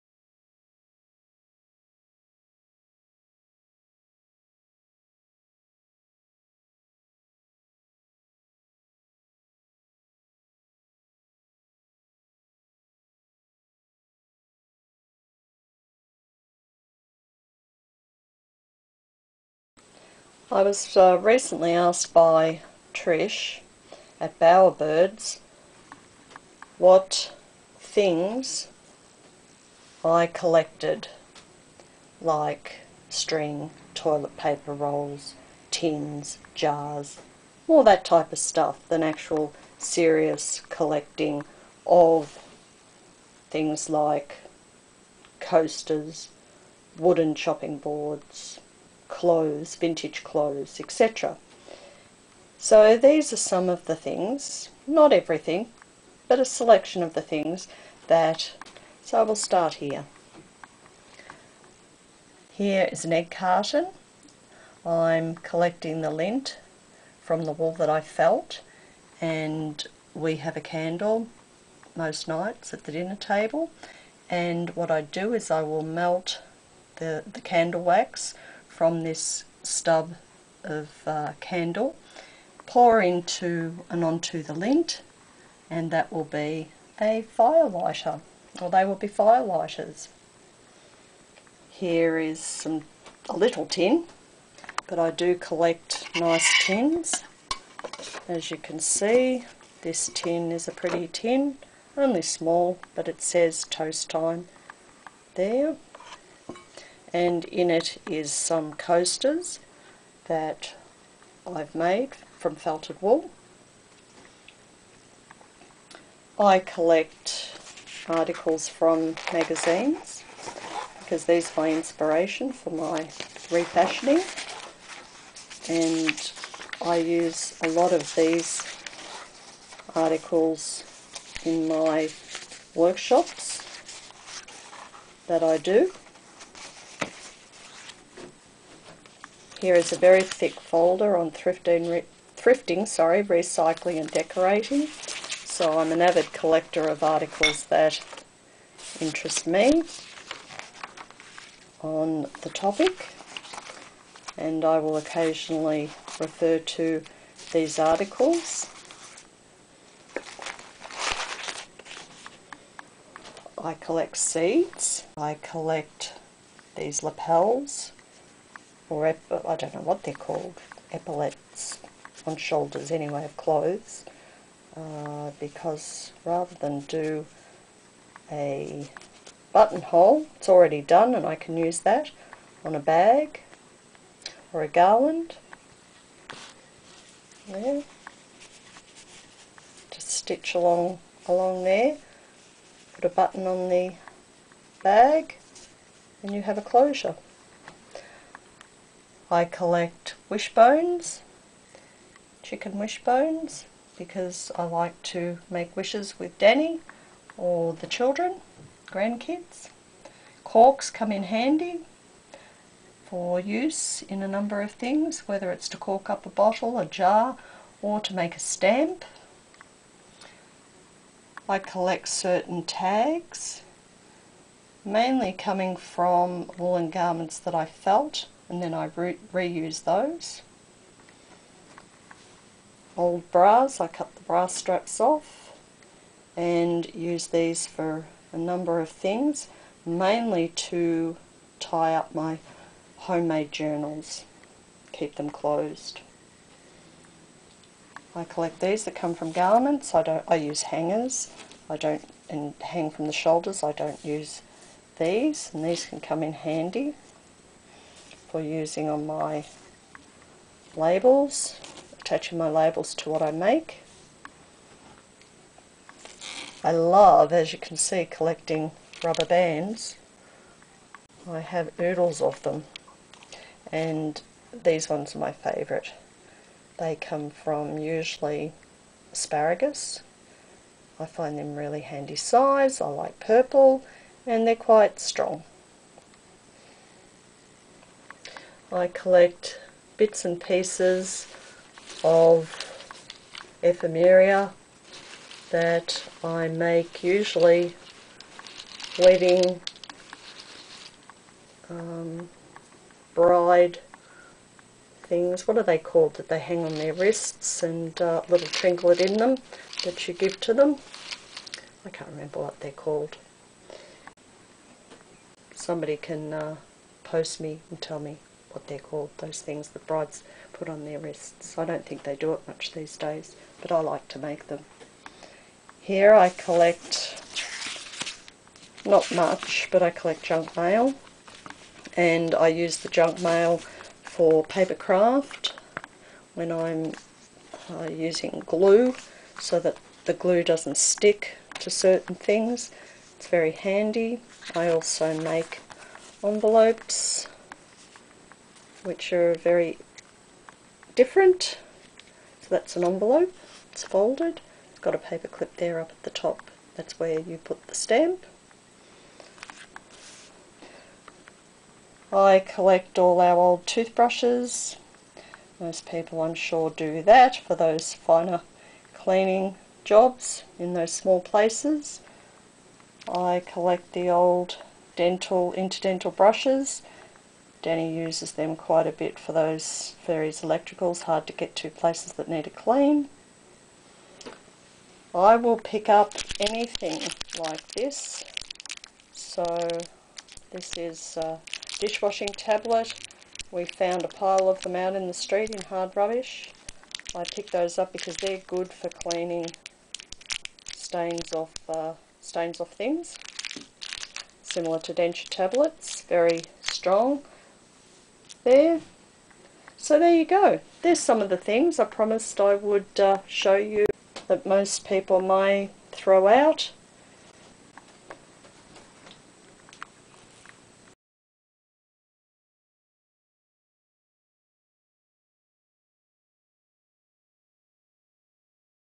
Karen's Saved Things
I like finding and keeping useful and practical things.  I almost nearly like everything providing it has a place and purpose.  There is nothing better than looking at other people's things that's why I made this video.